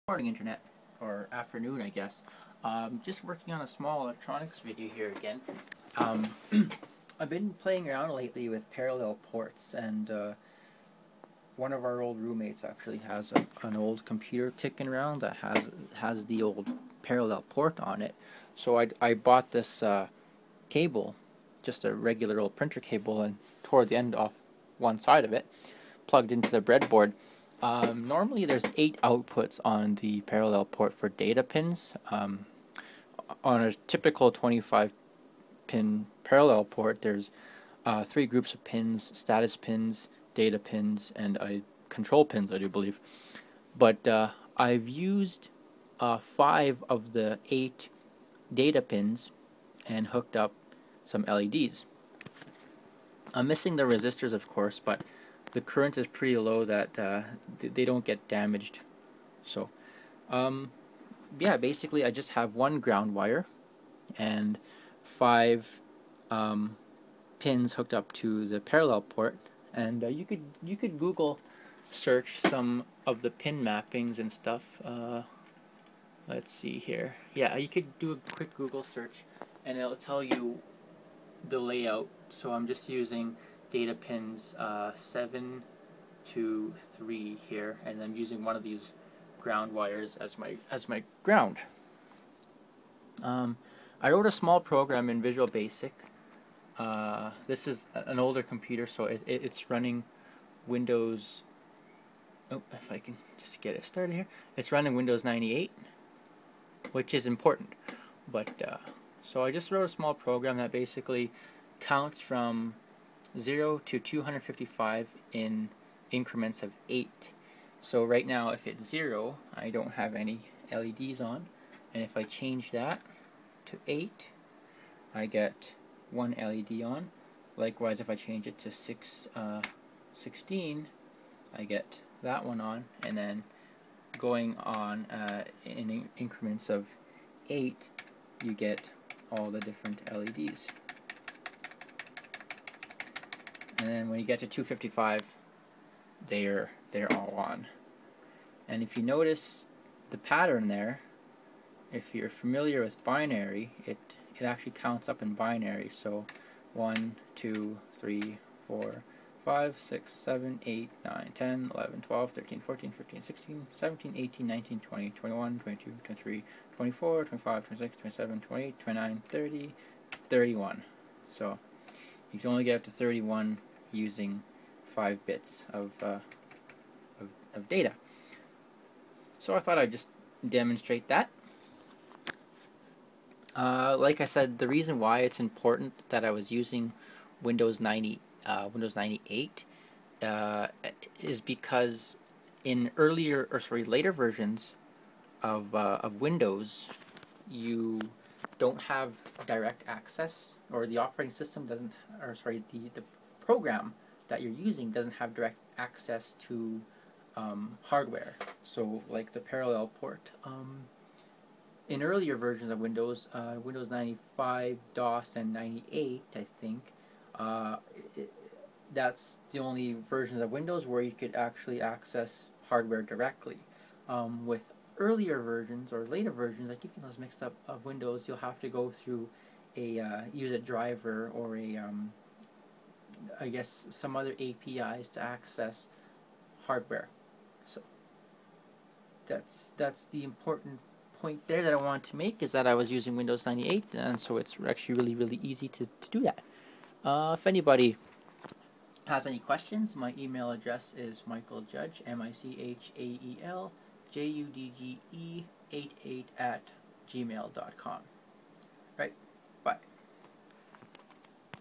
Using a PC Parallel Port to Control LEDs
Short demo of an old parallel port controlling LEDs.